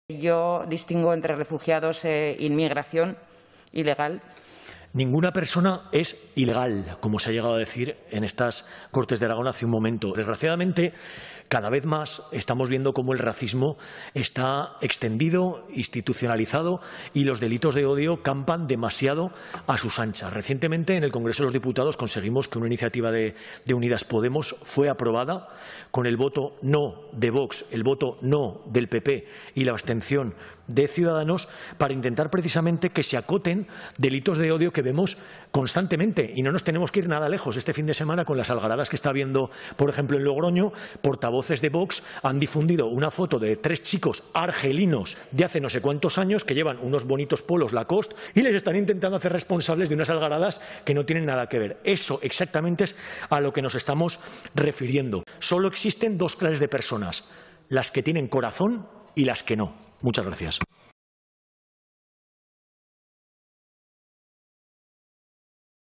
Le decimos BASTA YA al racismo y los delitos de odio de Vox
El racismo de la ultraderecha está haciendo un daño tremendo a la convivencia. Hoy en las Cortes de Aragón nuestro portavoz Nacho Escartín le ha dicho BASTA YA al discurso de odio de Vox en una comparecencia de Fundación Cepaim en la que han dado a conocer la bravísima labor que realizan en Aragón